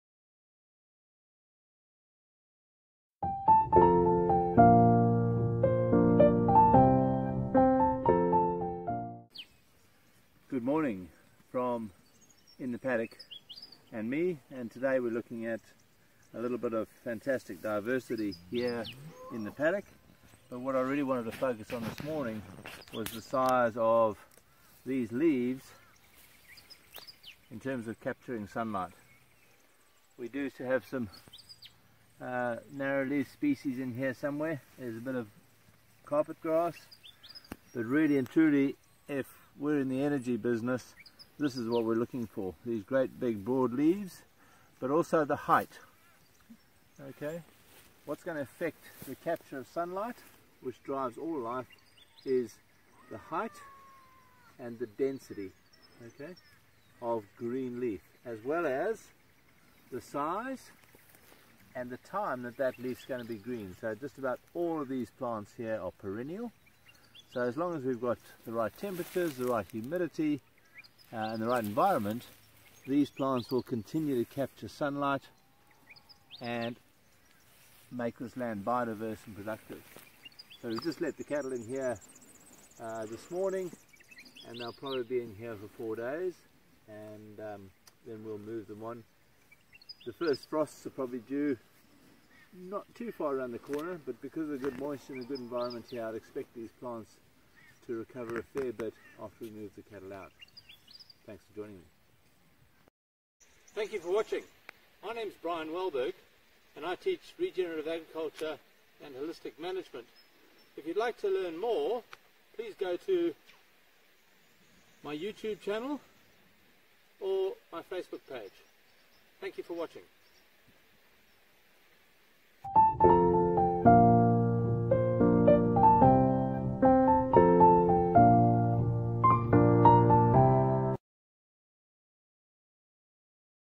1.6 Capturing Sunlight - The Paddock and Me
Capturing and packaging sunlight - into a carrot, an apple or a fillet steak, is the job of every land manager. Without green plants capturing sunlight through the photosynthetic process we could not have a bird, or a bee, or an elephant or a human!